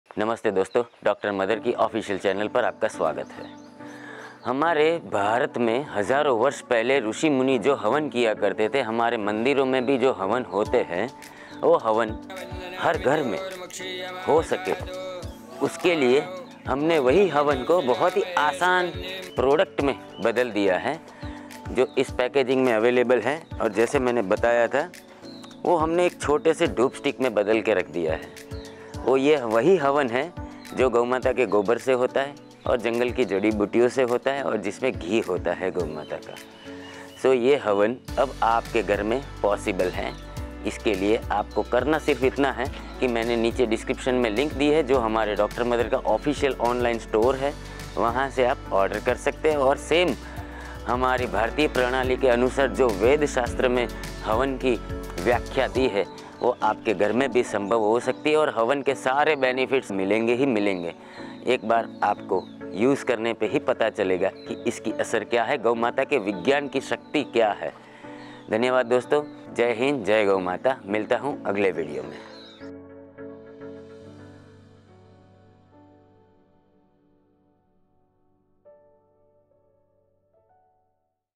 Doing vedic havan is now possible at home | भारतीय संस्कृति के अनुसार हवन करे रोजाना
one upon a time our rushimuni and sadhus in india always o the havan with Caw Gobar and forest herbs with the intention of air purity and generate positivity in the atmosphere, we are Dr.mother brand doing constant innovation to make it happen in every home to repeat the indian vedic culture.